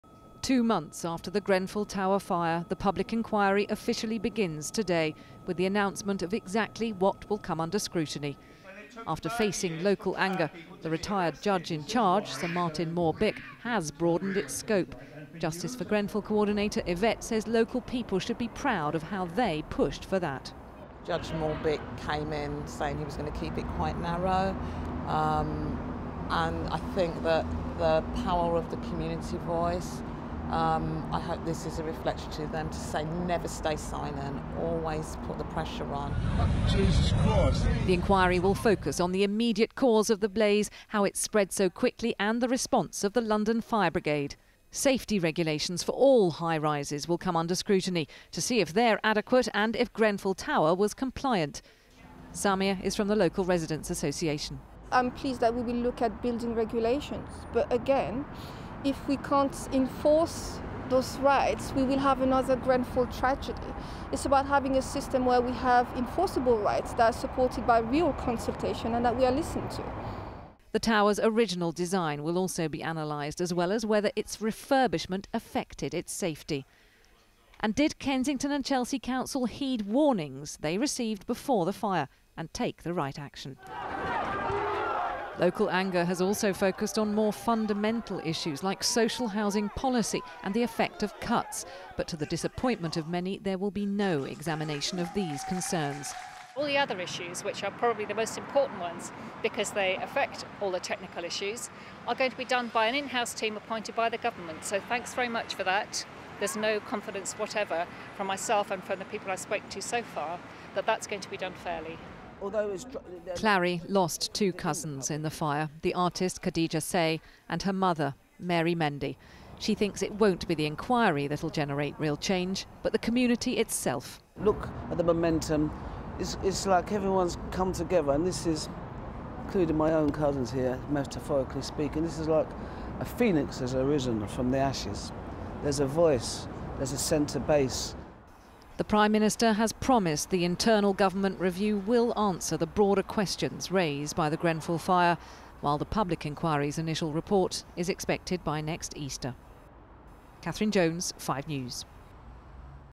Grenfell Tower: Anger as public inquiry into tragic blaze begins
The public inquiry into the Grenfell Tower fire, which killed at least 80 people in June, has officially started.  It'll look into the actions of both the Government and Kensington and Chelsea Council - as well as what caused the blaze, and whether building regulations are adequate.

But there's a lot of anger about what the inquiry WON'T cover.